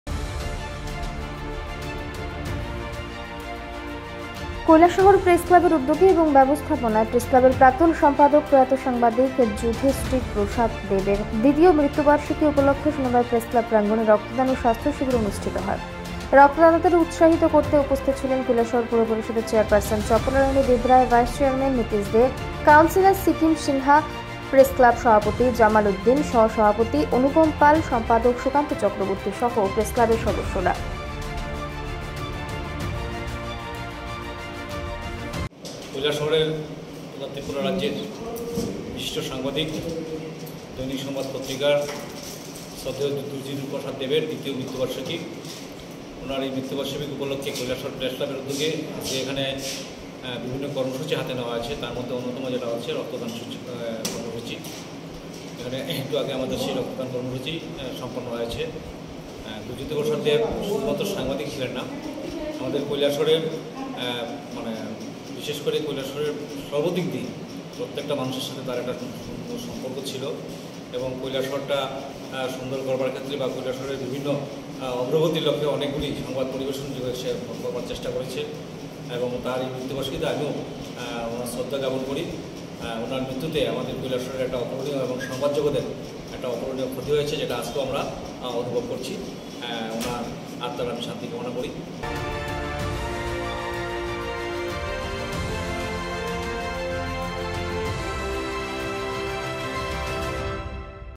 Kailashahar : কৈলাসহর প্রেসক্লাবের উদ্যোগে রক্তদান ও স্বাস্থ্য শিবির অনুষ্ঠিত || VOICE 18
VOICE 18
Name of Production House: FLAXEN MEDIA WORKS